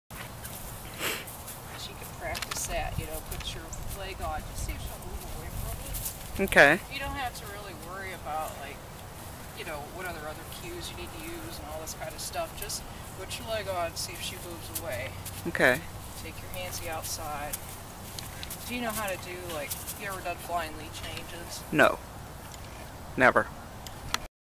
Week 5 under saddle 13th mount H
Working on leg aids, walk, trot, halt and baby side passes.